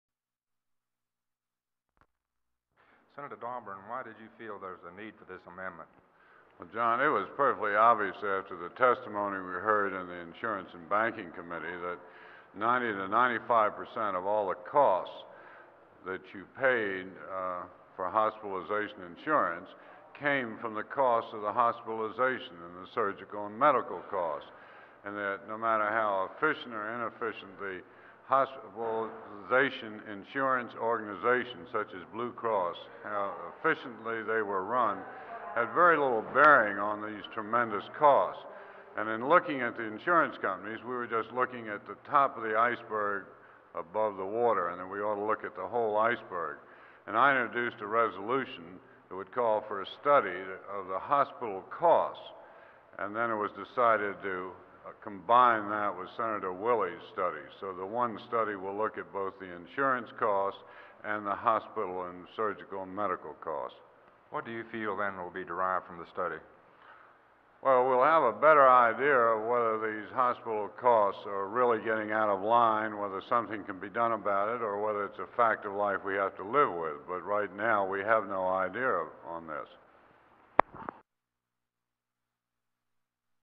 02/22/71 State Senator Buzz Dawbarn introduces a resolution that would called for a study of ho...
DAWBARN: 11 P.M. 2/22/71 JRS
THE STATE SENATE TOOK SOME ACTION TODAY IN THE FIELD OF HOSPITAL AND HEALTH INSURANCE. JOHN GILBERT TALKS WITH STATE SENATOR BUZZ DAWBARN ABOUT THE ACTION:
(TAKE SOUND FILM) (SOUND UP: 1:19
(SUPER: DAWBARN) (OUT CUE: IDEA ON THIS.